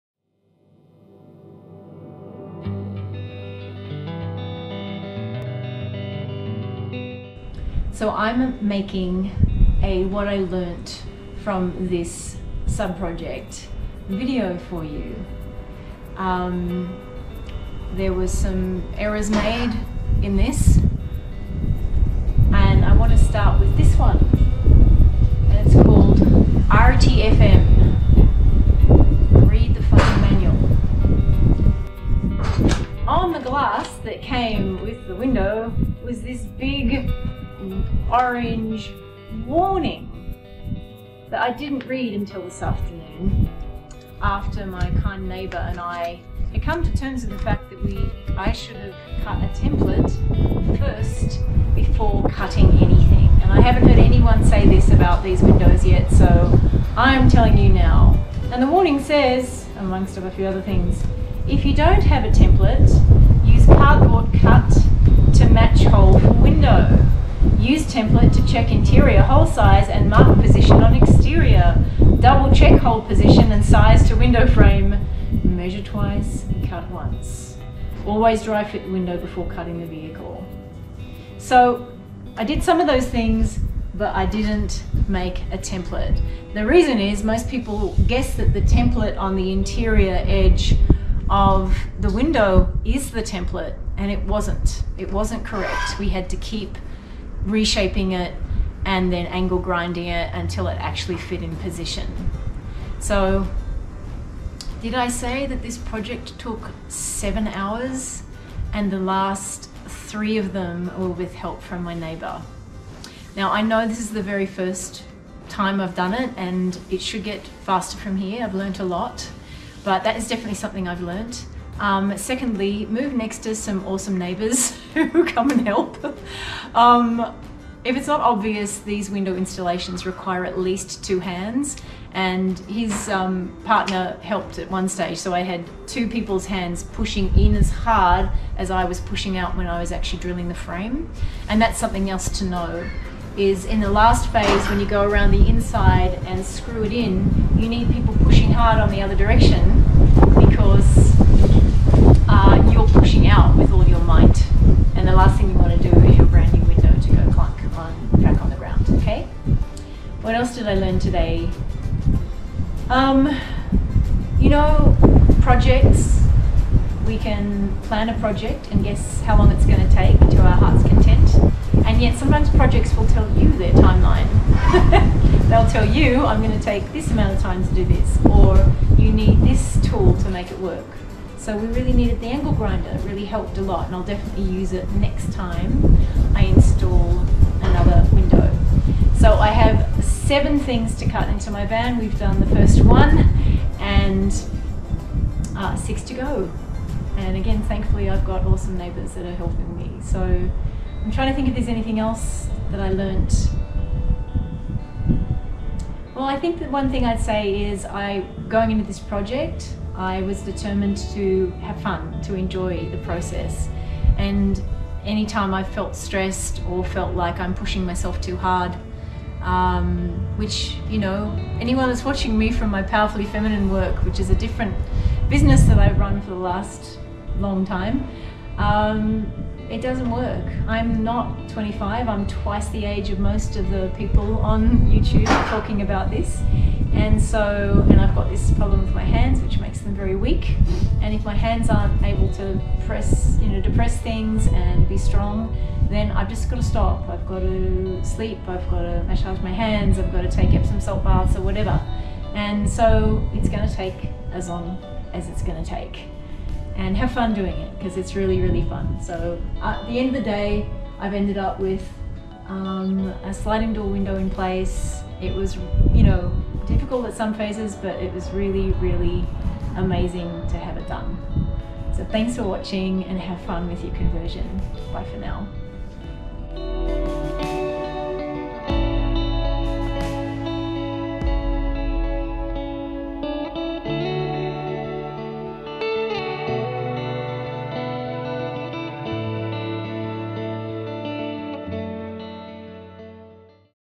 How To NOT Install CR Laurence Sliding Door Window | Van Build Series | Ep. 4c
Today I'm going to tell you what went wrong with my CR Lawrence vented sliding door installation in my Mercedes sprinter van.

BUT, I'll give you the biggest tip you'll need to ensure that you get it right, first go.